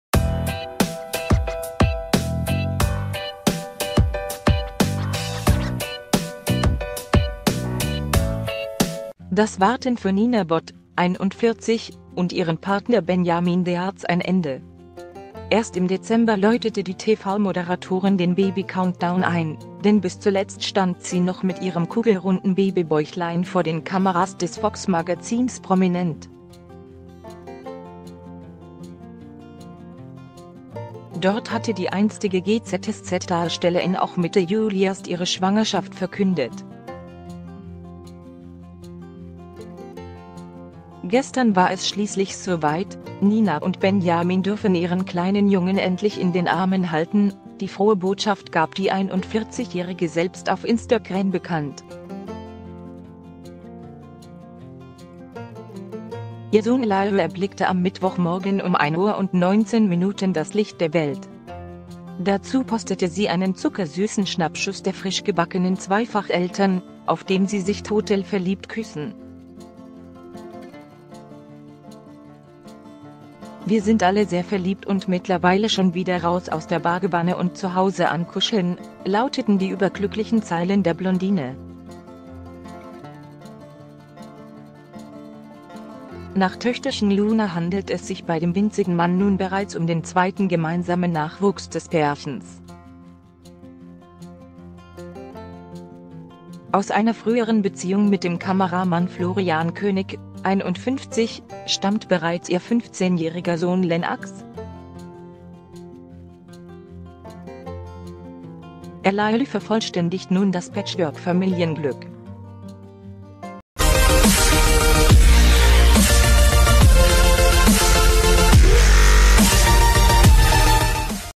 Es ist ein Sohn: Nina Bott ist zum dritten Mal Mama geworden - Aktuelle Nachrichten
Es ist ein Sohn: Nina Bott ist zum dritten Mal Mama geworden - Aktuelle Nachrichten  Das Warten für Nina Bott (41) und ihren Partner Benjamin Baarz ein Ende! Erst im Dezember läutete die TV-Moderatorin den Baby-Countdown ein, denn bis zuletzt stand sie noch mit ihrem kugelrunden Baby-Bäuchlein vor den Kameras des Vox-Magazins "Prominent". Dort hatte die einstige GZSZ-Darstellerin auch Mitte Juli erst ihre Schwangerschaft verkündet. Gestern war es schließlich so weit: Nina und Benjamin dürfen ihren kleinen Jungen endlich in den Armen halten!Die frohe Botschaft gab die 41-Jährige selbst auf Instagram bekannt. Ihr Sohn Lio erblickte am Mittwochmorgen um 01:19 Uhr das Licht der Welt. Dazu postete sie einen zuckersüßen Schnappschuss der frischgebackenen Zweifach-Eltern, auf dem sie sich total verliebt küssen. "Wir sind alle sehr verliebt und mittlerweile schon wieder raus aus der Badewanne und Zuhause am Kuscheln, lauteten die überglücklichen Zeilen der Blondine.Nach Töchterchen Luna handelt es sich bei dem winzigen Mann nun bereits um den zweiten gemeinsamen Nachwuchs des Pärchens. Aus einer früheren Beziehung mit dem Kameramann Florian König (51) stammt bereits ihr 15-jähriger Sohn Lennox. Lio vervollständigt nun das Patchwork-Familienglück.